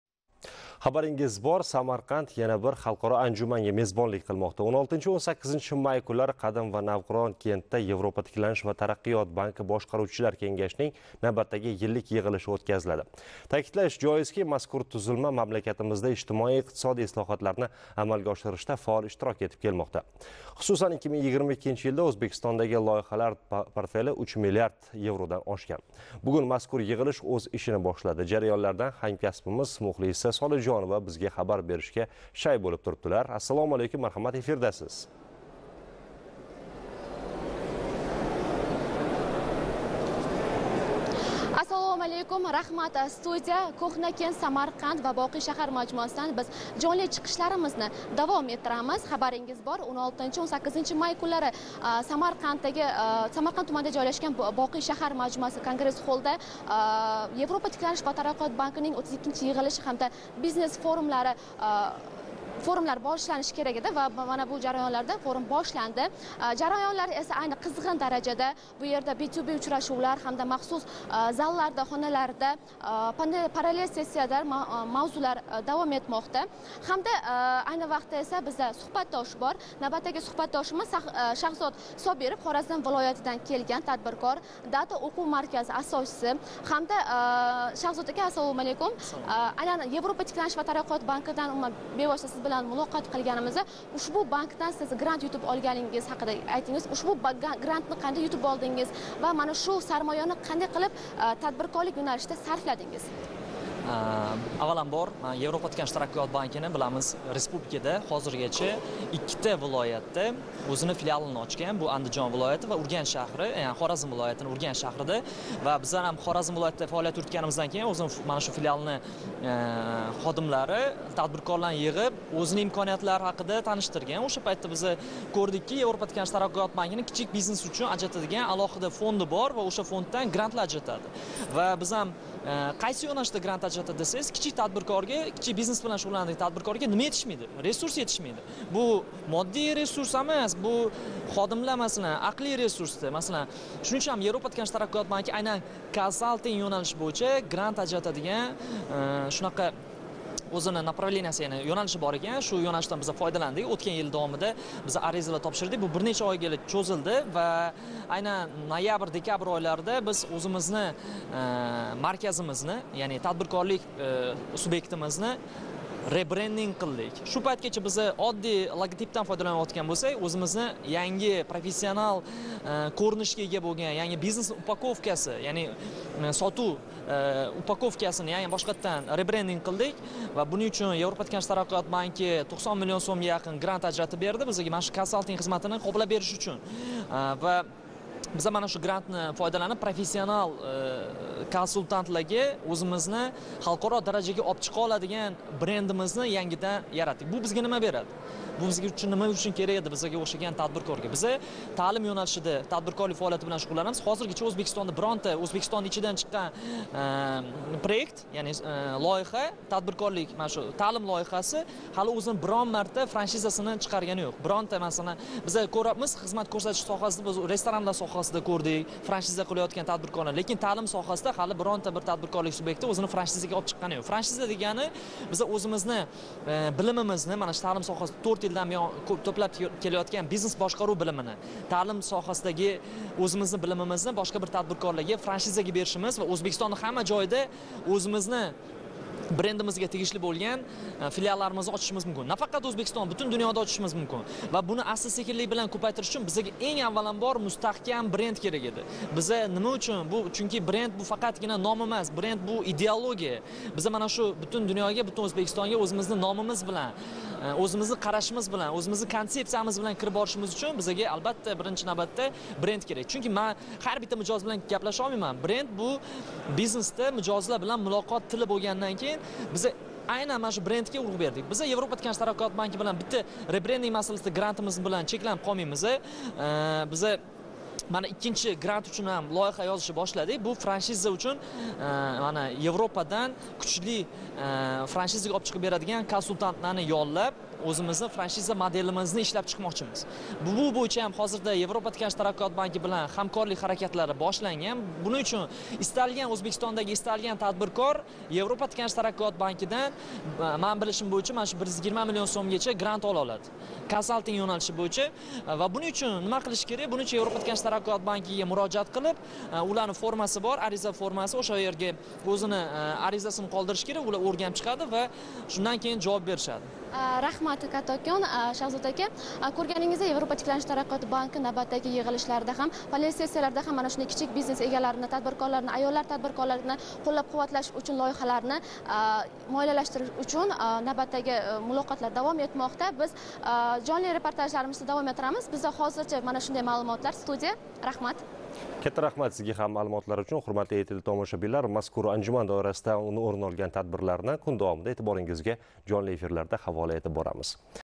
🇺🇿 “O’zbekiston24”
🇺🇿 “O’zbekiston24” telekanalida Samarqandda “Yevropa tiklanish va taraqqiyot” banki Boshqaruvchilar kengashining yillik yig‘ilishi yoritildi

U yerda stansiyamiz rahbari Shahzod Sobirov bankdan qaysi yo’nalish bo’yicha grant yutgani va uni nimaga yo’naltirgani haqida intervyu davomida aytib o’tdi.